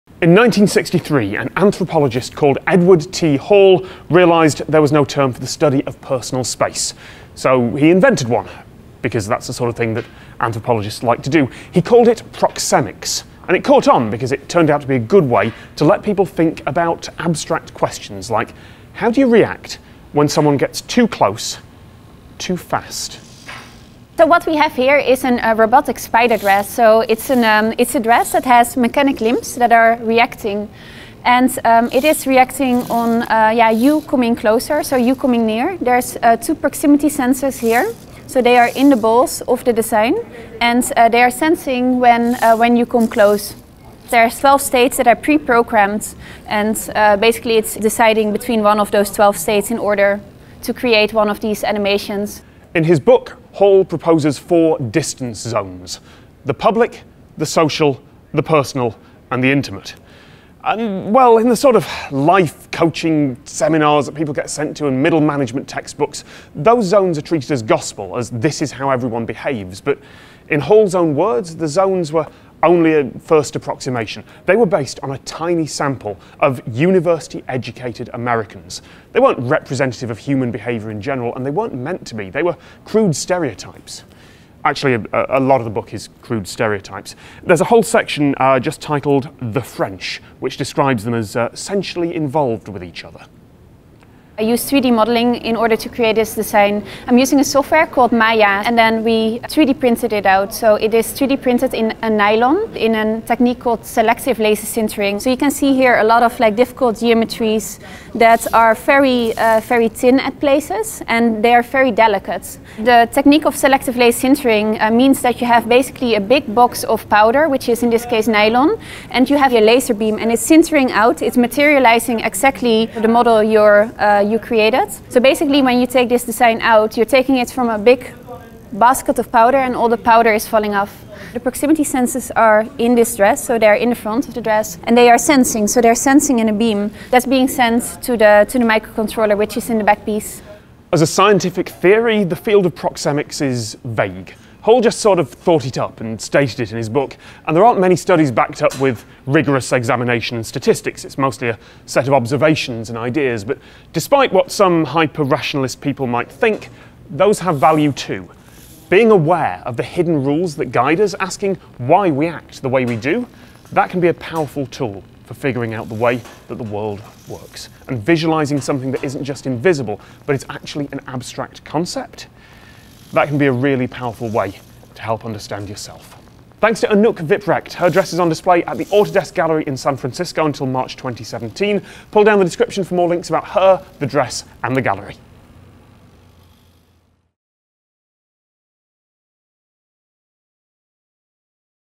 The Spider Dress That Reacts To Personal Space Invaders
Fashion-tech designer Anouk Wipprecht has built a Spider Dress, which reacts based on how close you're standing and how quickly you approached. It's based on 'proxemics': the study of personal space... although how much of that counts as science is an open question. Let's talk about Edward T Hall, about what counts as science, and what happens if you get too close to someone.

Anouk's dress is on display at the Autodesk Gallery in San Francisco until March 2017

FULL DISCLOSURE: Autodesk were good enough to cover my travel to San Francisco, but they haven't paid me and had no control over the script, the content or the final cut!

BIBLOGRAPHY:
Hall, E. (1963). A System for the Notation of Proxemic Behavior. American Anthropologist, 65(5), pp.1003-1026.
Hall, E. (1966). The hidden dimension. Garden City, N.Y.: Doubleday.

Camera by Andy Westhoff

and on Instagram and Snapchat as tomscottgo